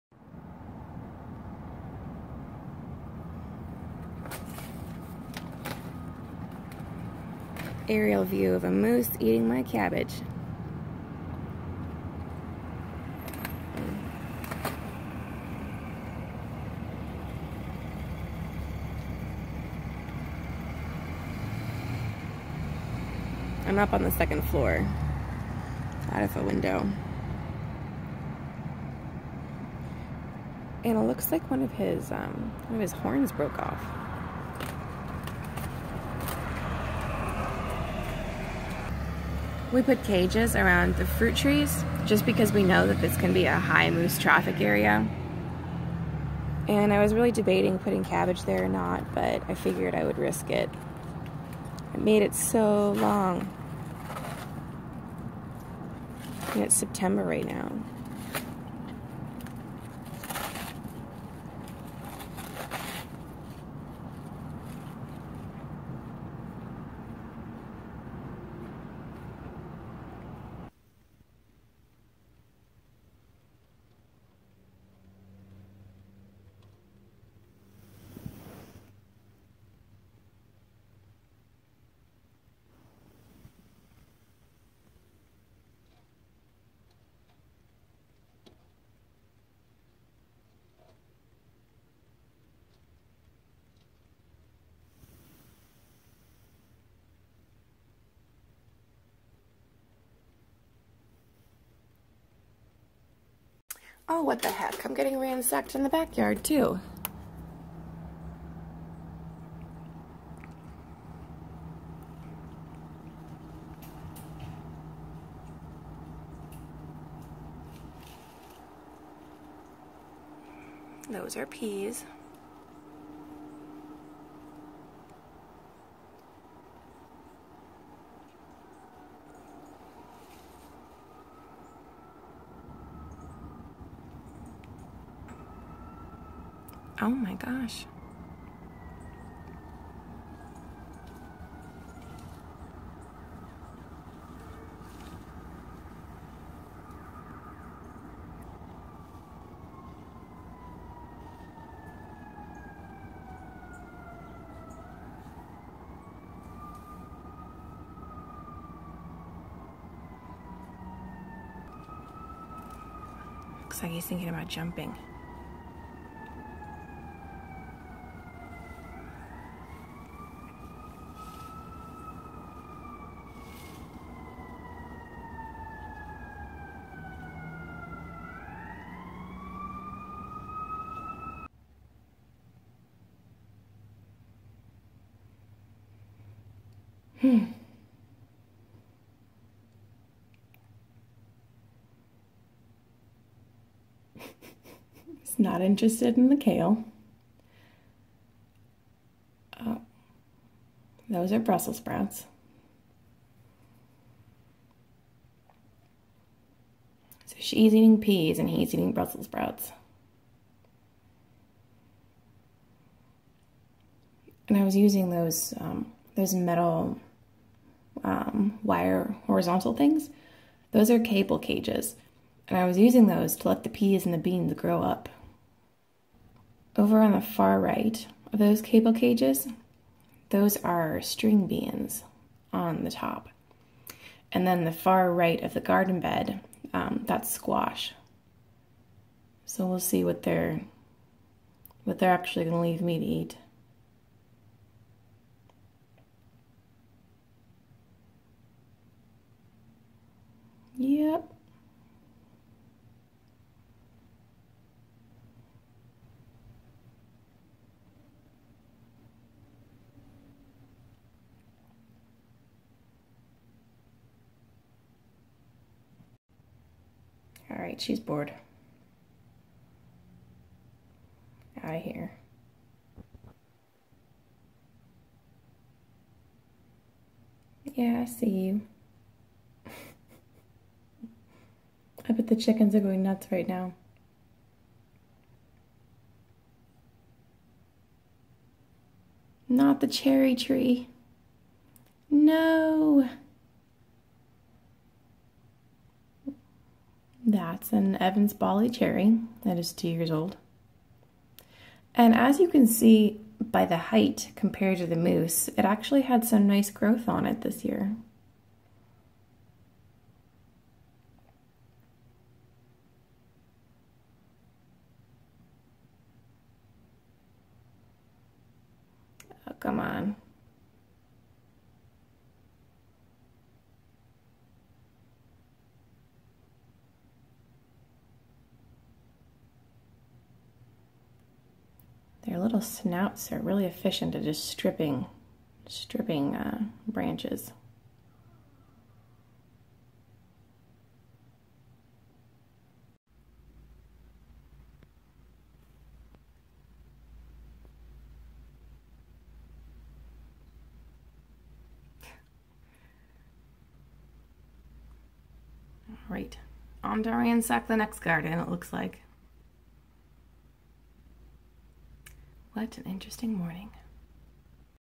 Cabbage for breakfast? Two moose munch on garden beds
I looked out the window this morning just in time to see a young male moose munching on my cabbage garden bed. He had something wrong with his left horn. It looked broken, like it had snapped off, but was still hanging by the side of his head.

I looked in the backyard next, to admire my garden that was still standing, when I saw a second moose in the backyard. I got a few shots of them jumping over the chain link fence, and the younger male moose stripping a 2-year-old Evans Bali cherry tree. Overall, it was a very entertaining and eventful morning here in Anchorage, Alaska!